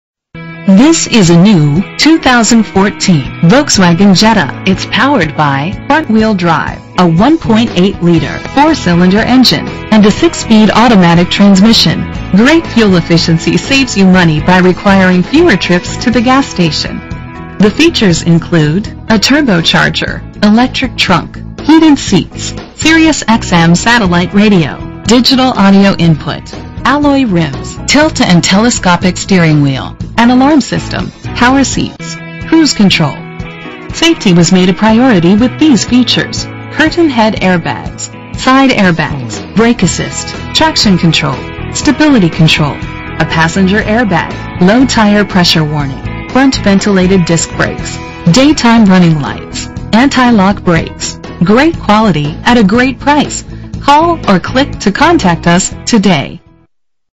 2014 Volkswagen Jetta - Huntsville AL
This vehicle represents similar vehicles available at Hiley Mazda VW Audi.
for availability of specific vehicles.

Phone:
Year: 2014
Make: Volkswagen
Model: Jetta
Trim: SE w/Connectivity
Engine: 1.8 liter inline 4 cylinder 16 valve GDI DOHC Turbo
Transmission: 6-Speed Automatic
Color: Pure White
Mileage: 5
Address:
6561 University Drive